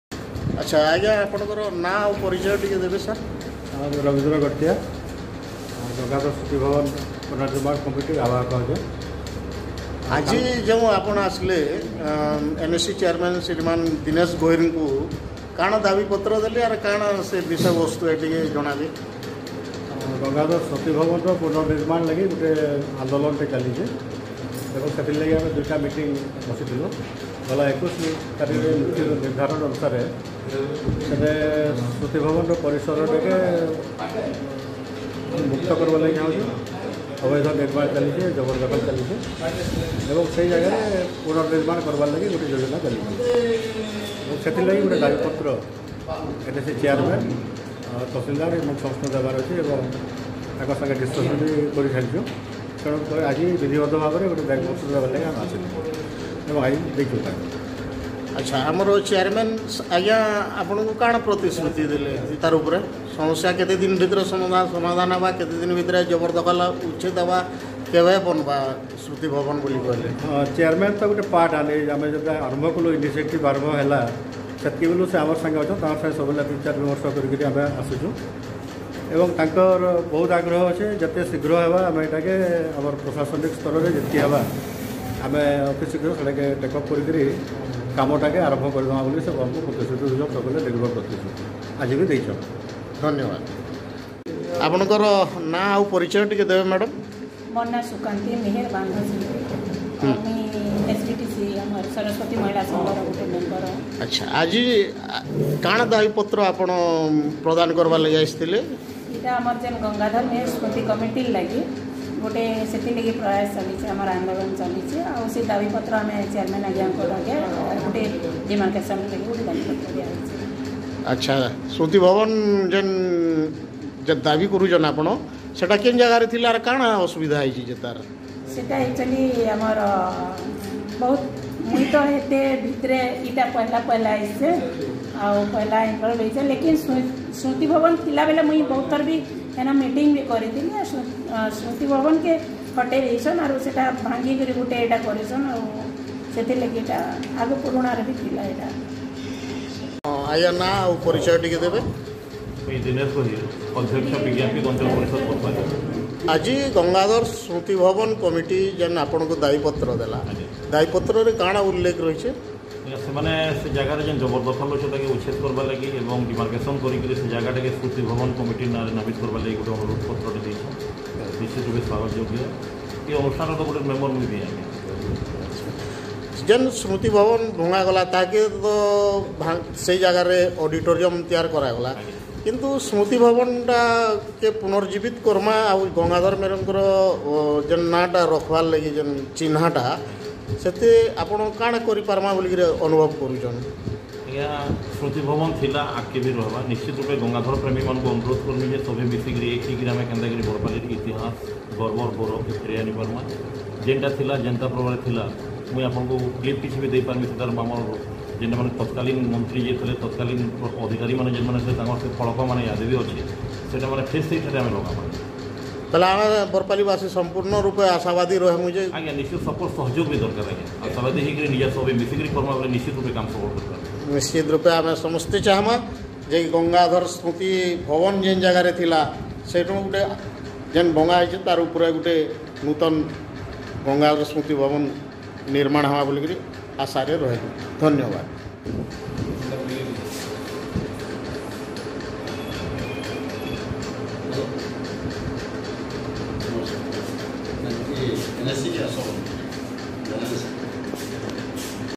गंगाधर स्मृति भवन पुनर्निर्माण आंदोलन: तहसीलदार सहित NAC अध्यक्ष को दिया गया मांग पत्र!!
गंगाधर स्मृति भवन के पुनर्निर्माण के लिए बरपाली में चल रहा है नागरिकों का आंदोलन!!

इस सिलसिले में कल बरपाली के तहसीलदार को एक मांग पत्र देकर डीमारकेशन करने के लिए कहा गया! इसके साथ ही पूरी ज़मीन गंगाधर स्मृति भवन के नाम पर रजिस्टर करने की भी उचित मांग की गई!!

गंगाधर स्मृति भवन परिसर से सारे अतिक्रमण, अवैध निर्माण को तोड़ने की भी मांग हुई!!

आज बरपाली के NAC अध्यक्ष को भी मांग पत्र देकर गंगाधर स्मृति भवन शीघ्र बनाने की मांग की गई!!

(यह वीडियो बरपाली निवासी और समाज सेवी धर्मराज सिंह बरिहा के सौजन्य से)